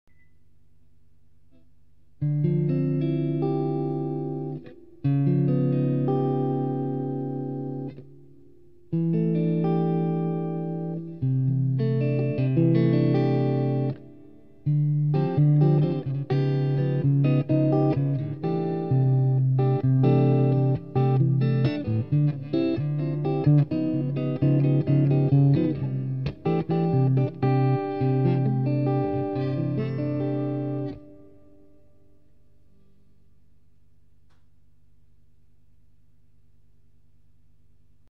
Dm9-(Trit)C69.mp4
for explanation and lots more info.

Another basic II-V(tritone)-I cadence showing the tritone substitution.